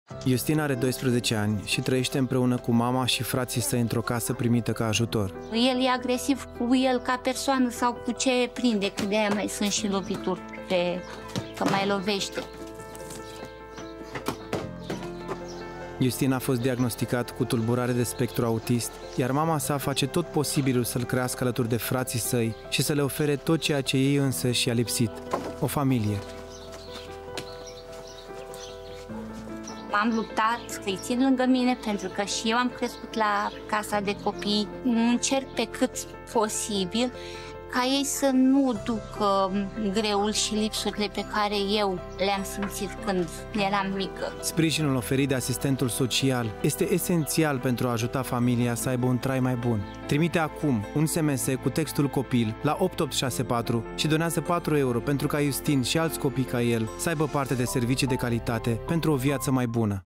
Ajută-i pe Iustin și pe alți copii ca el!
Iustin a fost diagnosticat cu tulburare de spectru autist, iar mama lui face tot posibilul să îl crească alături de frații săi și să le ofere tot ceea ce ei înseși i-a lipsit.

Sprijinul oferit de asistentul social este esențial pentru a ajuta familia să aibă un trai mai bun.

Donează online pe unicef.ro pentru ca Iustin și alți copii ca el să aibă parte de servicii de calitate.